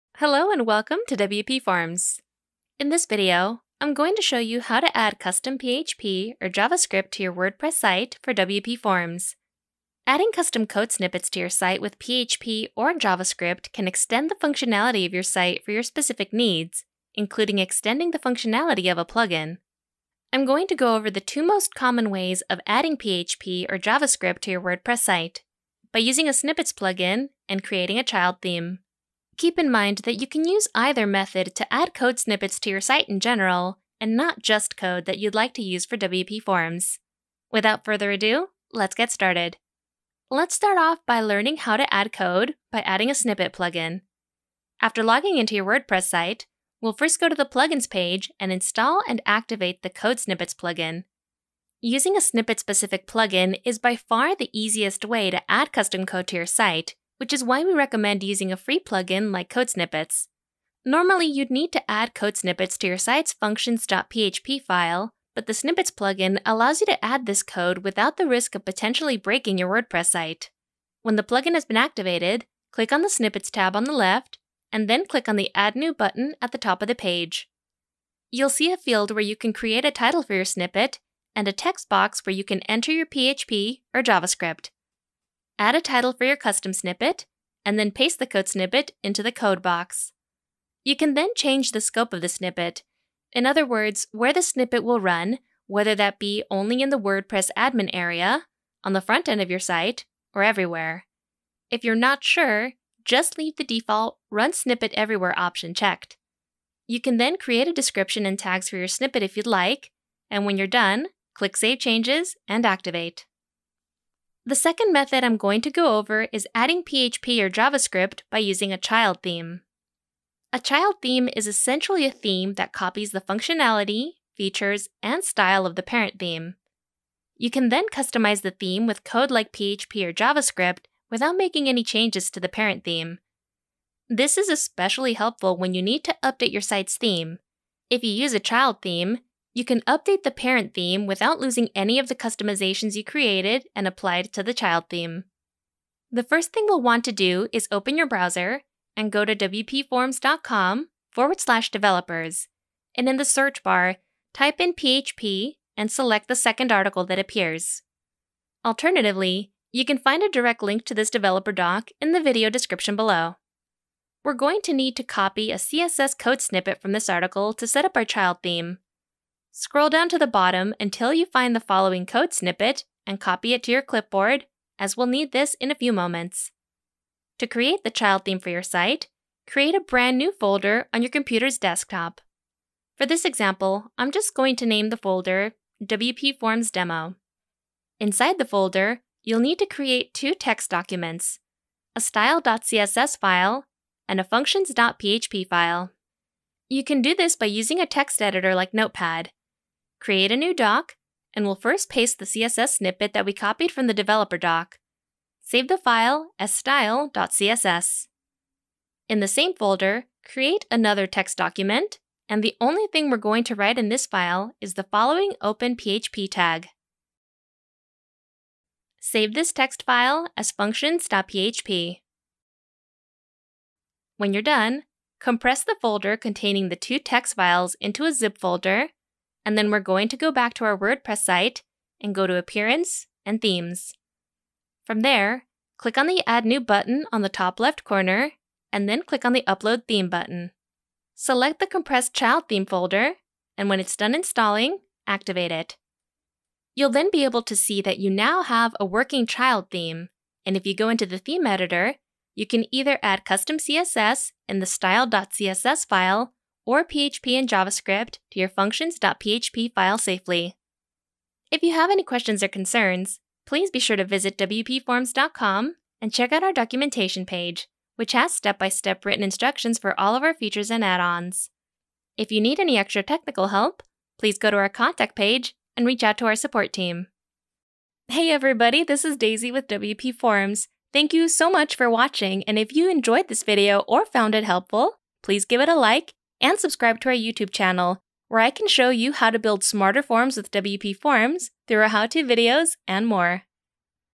How to Add Custom PHP or JavaScript to WordPress
Welcome to WPForms!

In this video, I'm going to show you how to add custom PHP or JavaScript to your WordPress site!

Adding custom code snippets to your site with PHP or JavaScript can extend the functionality of your site for your specific needs, including extending the functionality of a plugin.